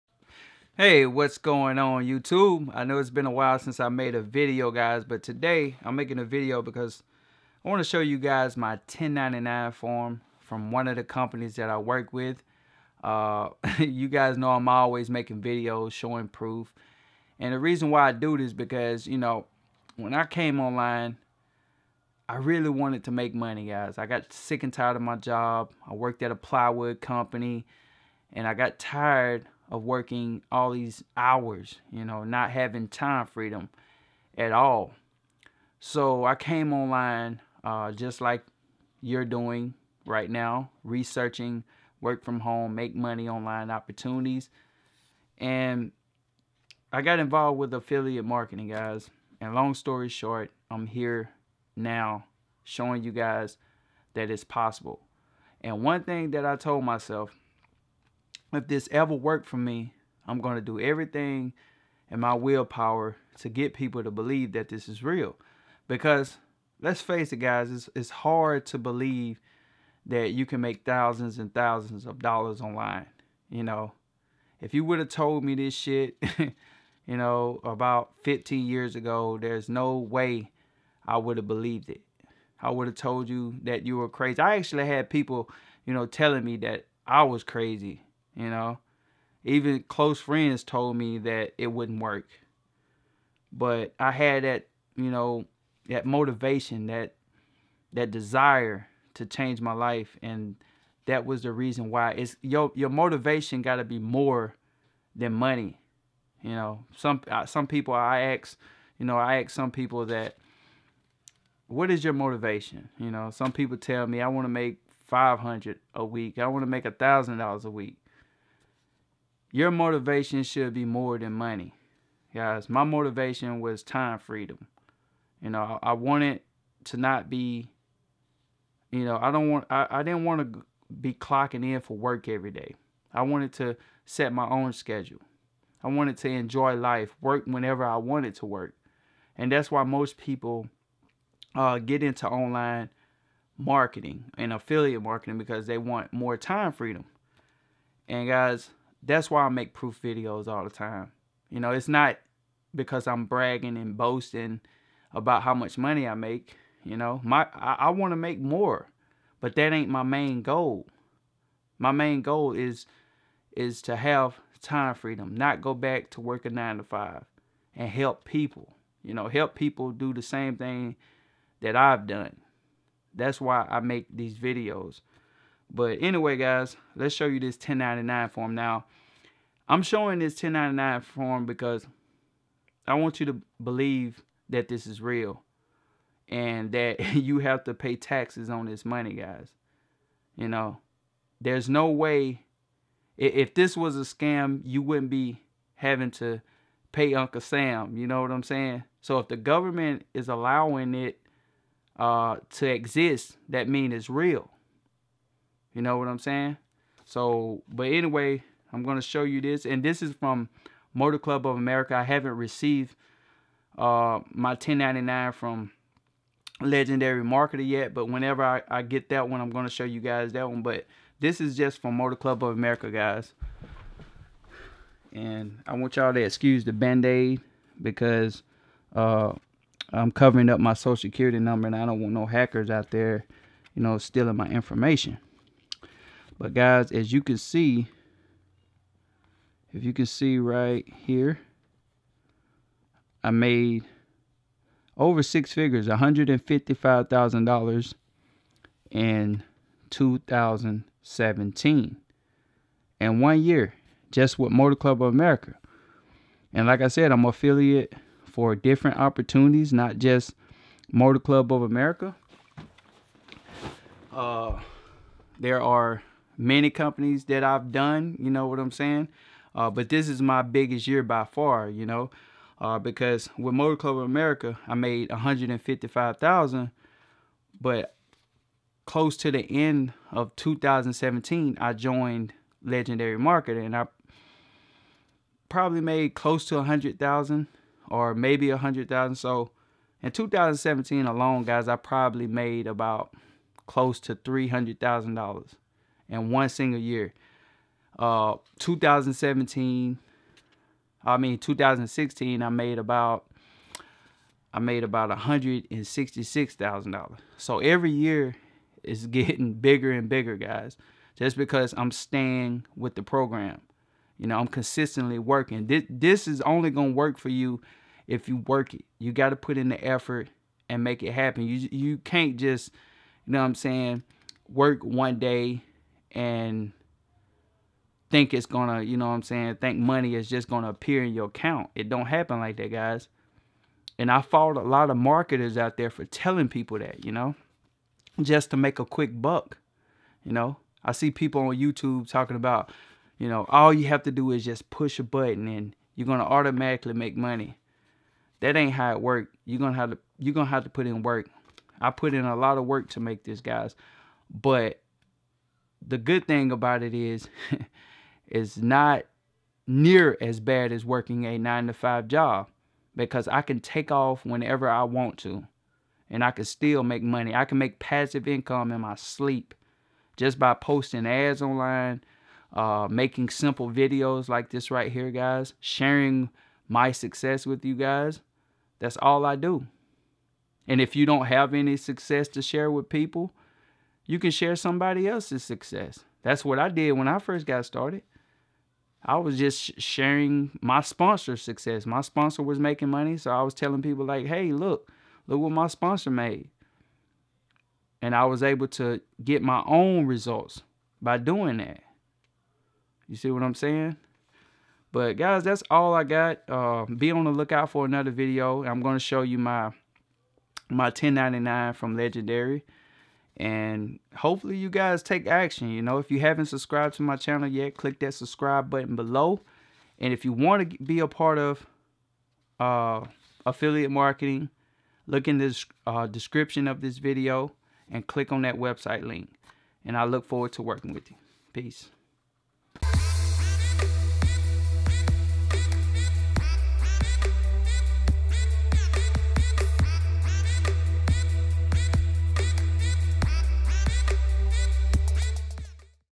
$155,149.80 per Year With Affiliate Marketing (This is How!)
How to do affiliate marketing

In this video I show you my 1099 form I received from MCA, just to show you that this is real and no fluff. If you have to pay taxes on your money that you make online then that means the company is legit right?

Anyone can do this no matter how old you are,  your race, where you work, as long as you have a computer with internet connection or even a tablet, or smartphone That's all you need then you can make money online as an affiliate marketer.

Affiliate marketing how to. Affiliate marketing for youtube

There are a lot of affiliate marketing programs out there but the one I recommend is above is description and below.